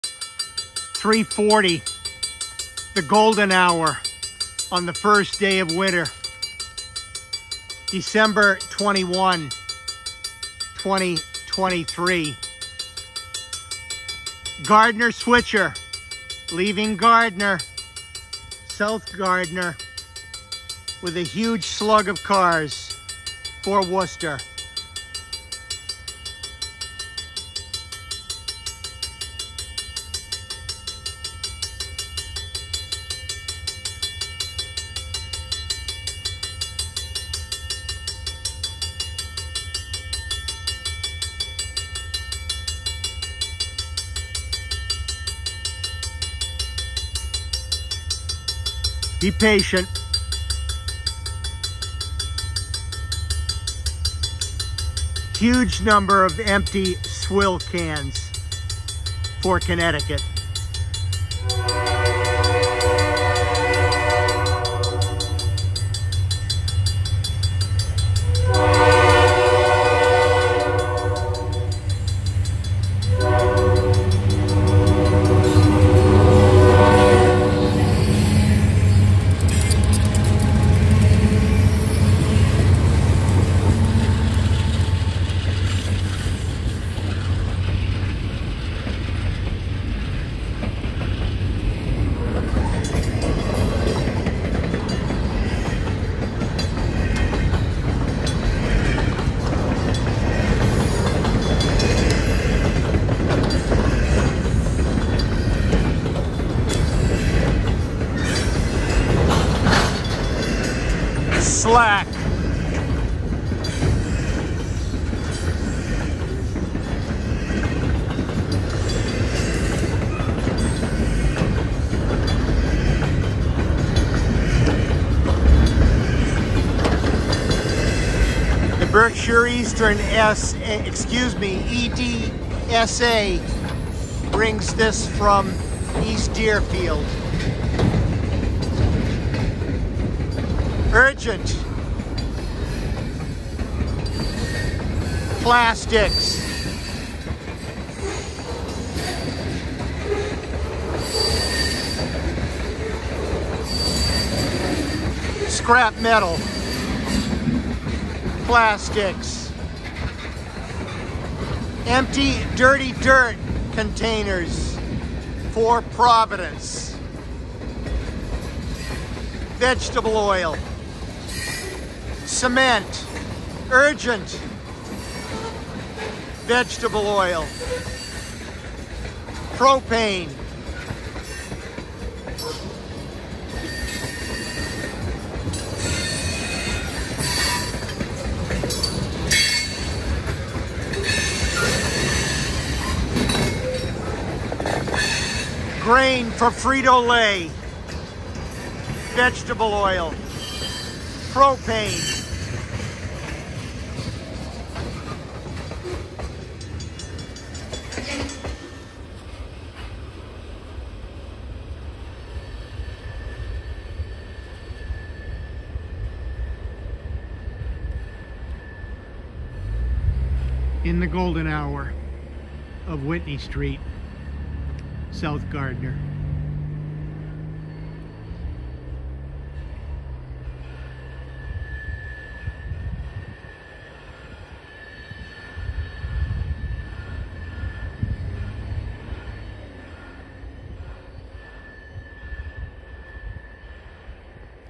Gardner Switcher W/ Friendly Engineer Runs Back to Worcester W/ HUGE # of Empty Swill Cans & Others
P&W Railroad Videos